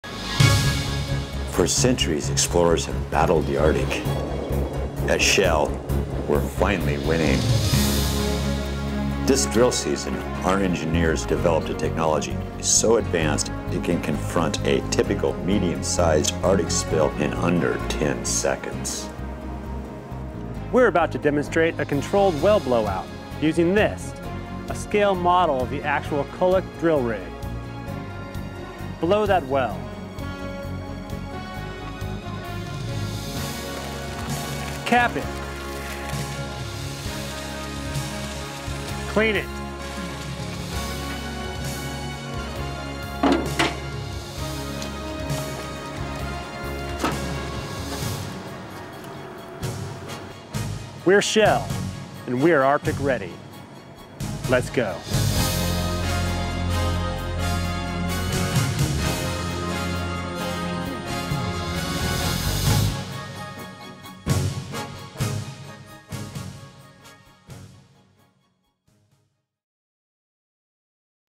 Arctic Oil Spill Response in Under 10 Seconds
This promotional video explains how Shell engineers can control and cap an Arctic well blowout in less than 10 seconds.